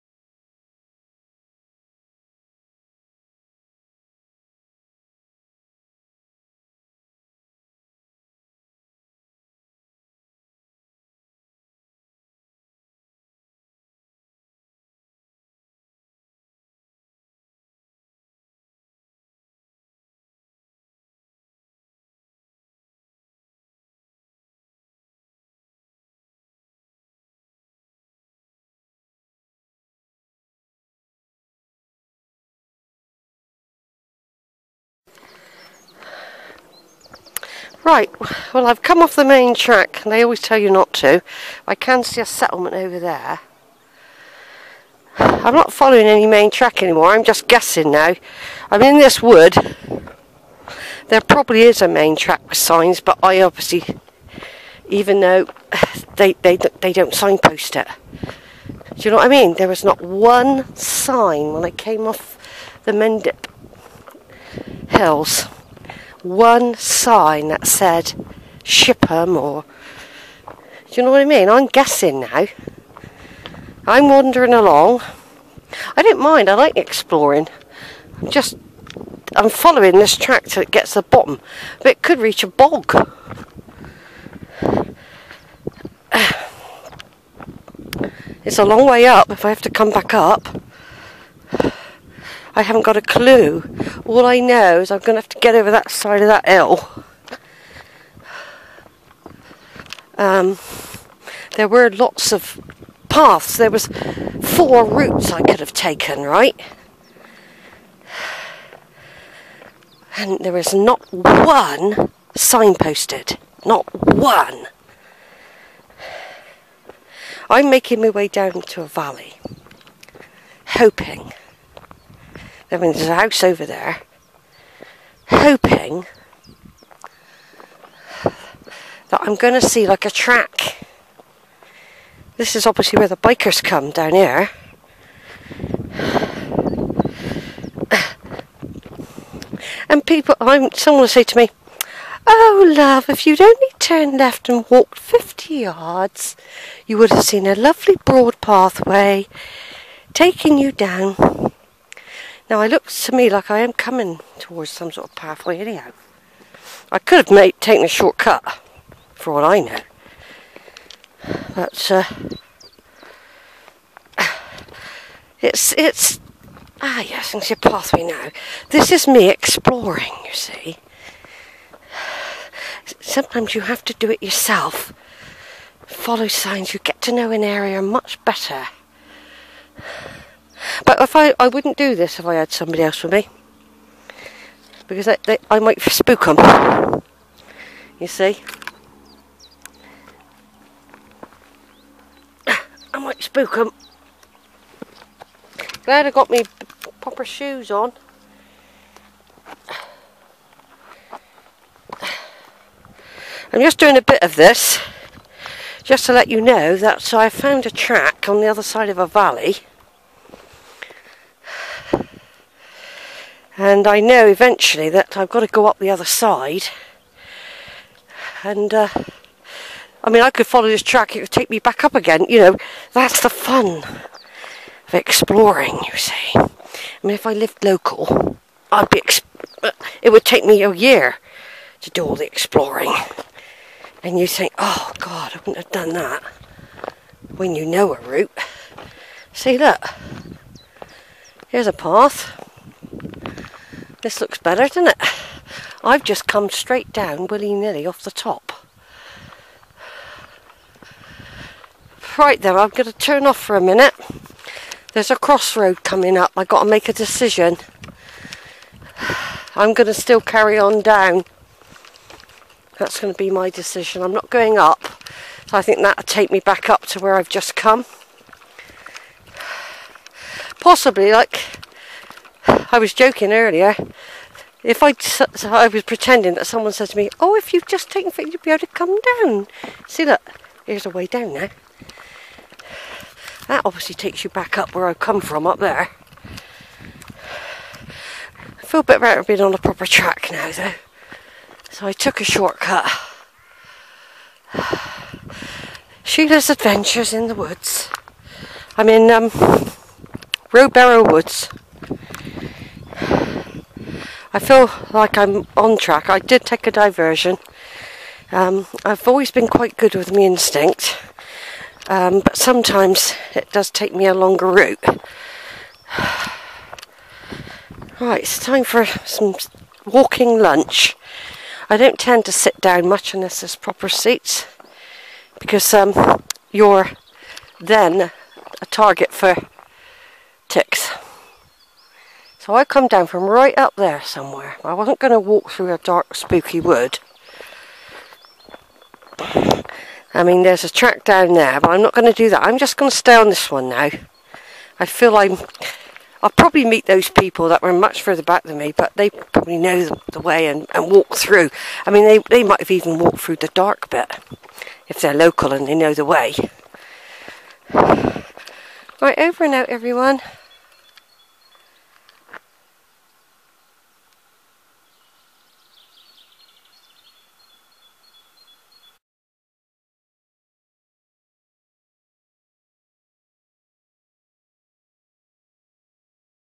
Cheddar to Shipham & Winscombe Walk, PART IX, 'Exploring Rowberrrow Warren', by Sheila, 17th April 2
After deciding to go off track I follow a mountain bike route down and down into the valley aware that I will need to climb up the other side at some point for Rowberrow and Shipham---I find a defined track and decide to pursue its course for a while